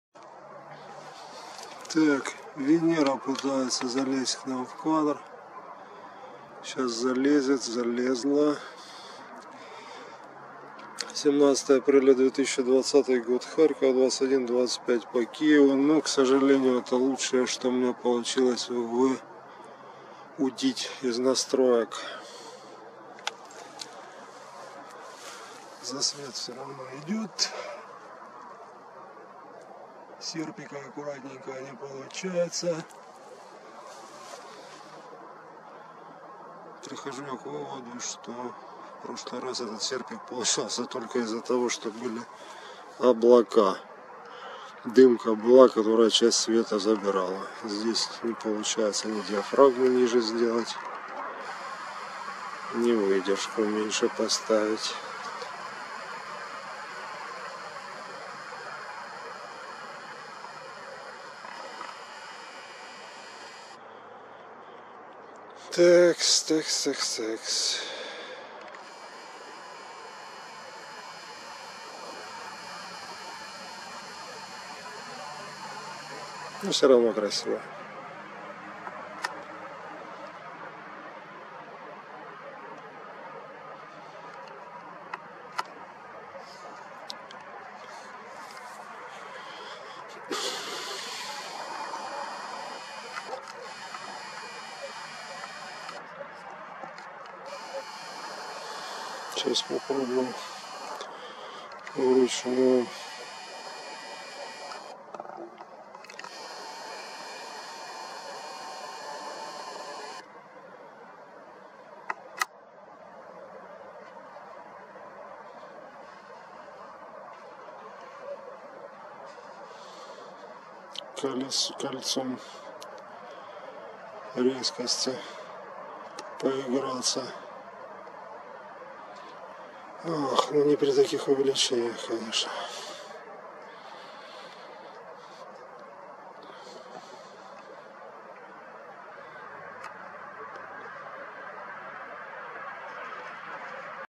Серп Венеры вползает в кадр 17 апреля 2020 Харьков
Венера над Харьковом 17 апреля 2020 - серп Венеры.